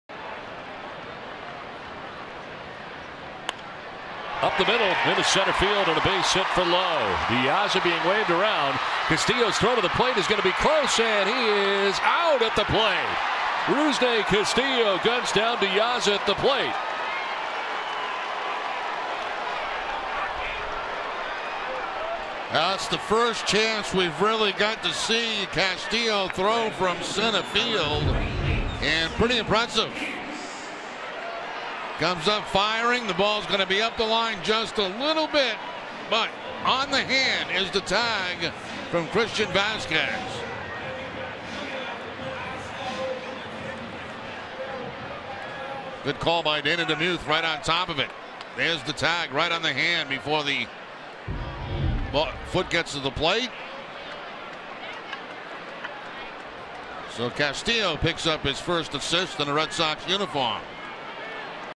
BOS@BAL: Castillo gets first assist with nice throw
9/19/14: Rusney Castillo makes a strong throw home, nabbing Alejandro De Aza for his first career assist